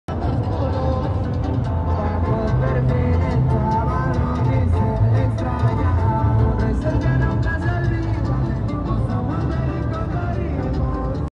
📹💀Devotos de la Santa Muerte se ponen “bélicos” con corridos de Peso Pluma.
Un contingente de devotos de la Santa Muerte salió a peregrinar por las calles del centro de Pachuca, como cada 2 de noviembre